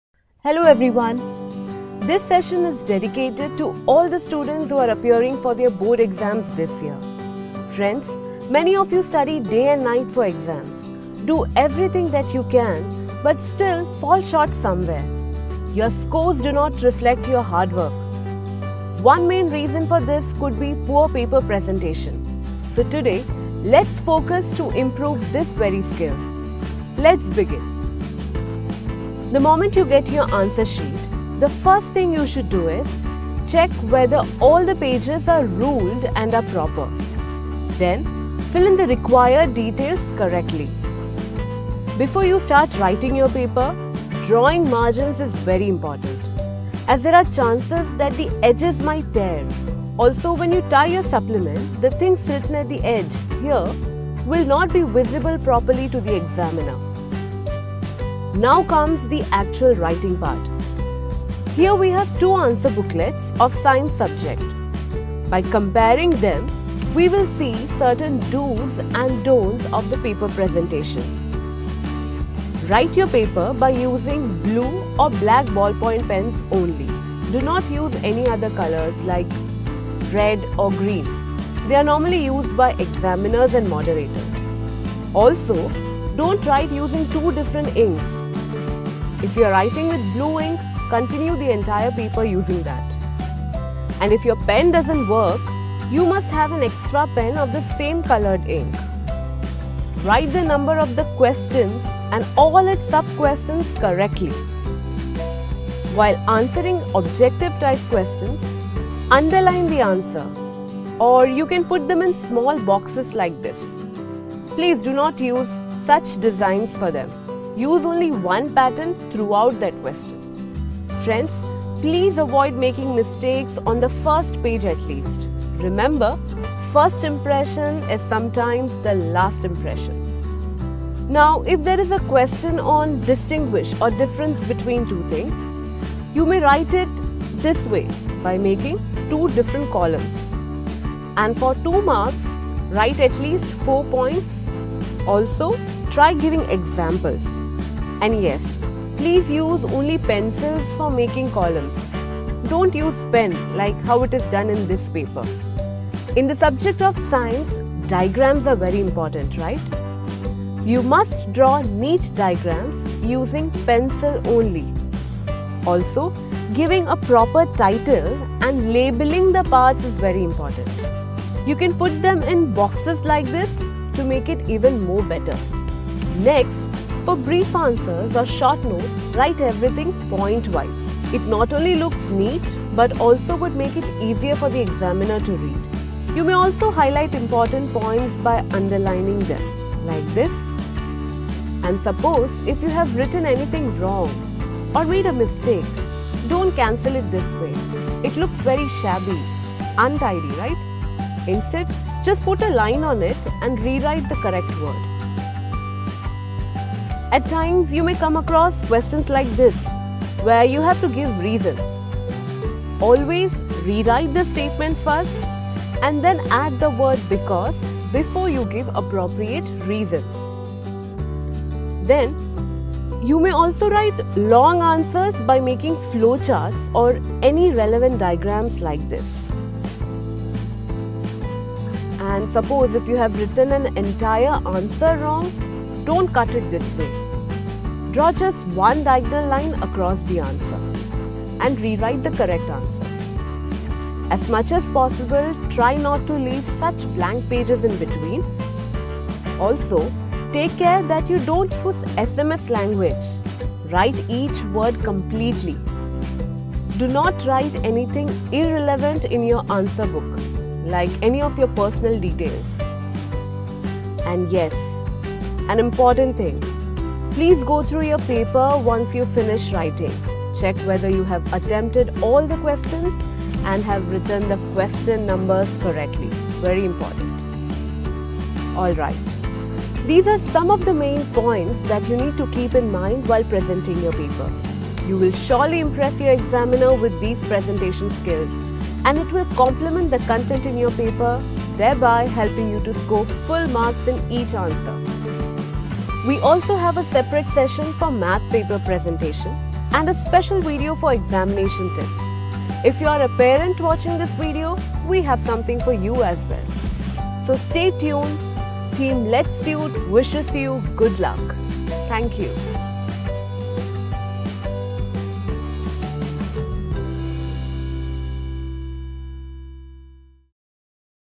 Std 10-12 Board Paper Presentation Tips For Students -
માત્ર પરીક્ષા આપવી જરૂરી નથી પણ પરીક્ષામાં કઈ રીતે રજૂઆત કરવી એવા અક્ષરો કાઢવા વિભાગ પહેલા લખવું તે વિશે પણ જાણકારી રાખવી ખૂબ જરૂરી છે વીડિયોમાં તમારી પ્રતિભા શક્તિ કેવી રીતે ખીલવવી જેથી તમારે સારા માં આવી શકે તેને સંપૂર્ણ જાણકારી છે